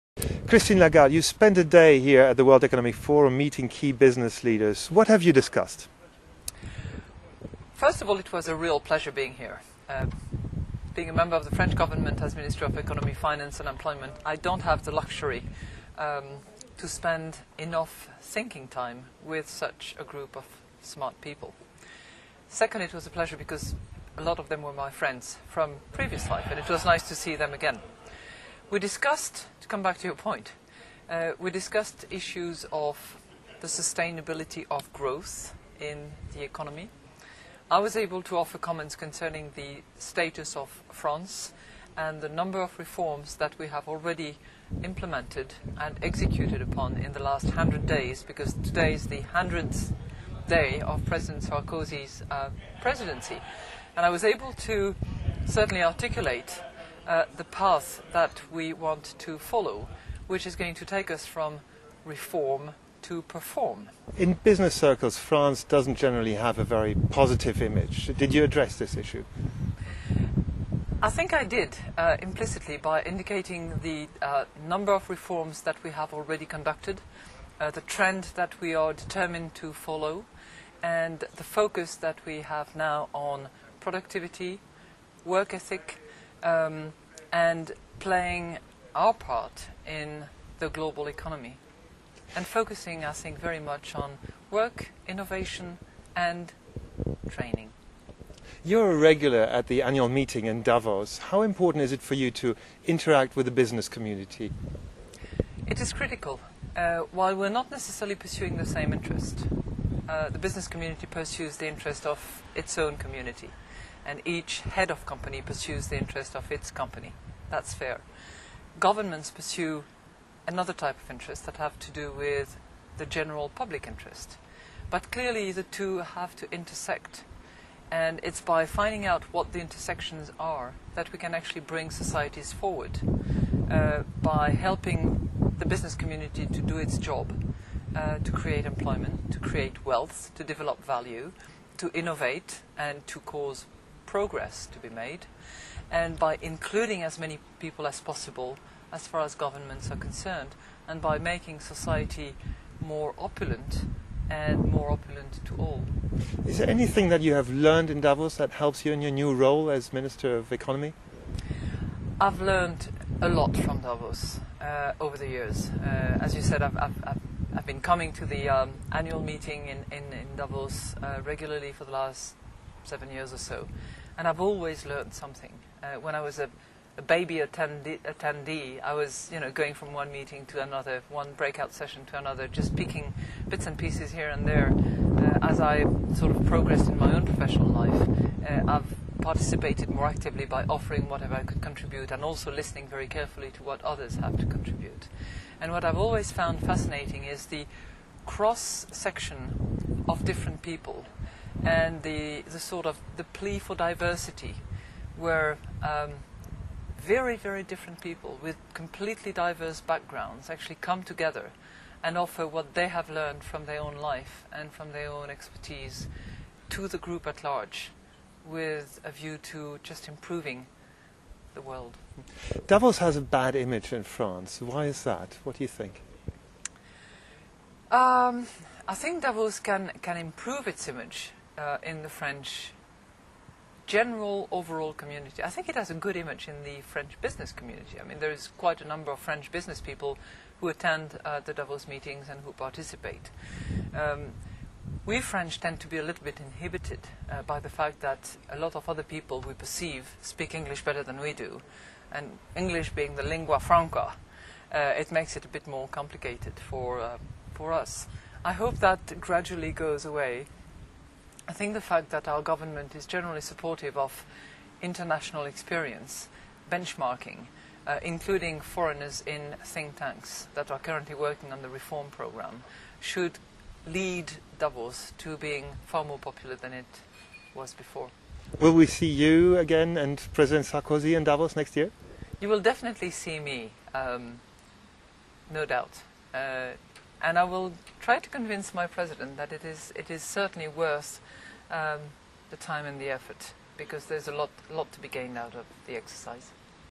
Christine Lagarde - France and the World Economic Forum
Christine Lagarde, Minister of Economy, Finance and Employment of France talks about France and the World Economic Forum after a private meeting with key business leaders at the Forum in Geneva, Switzerland.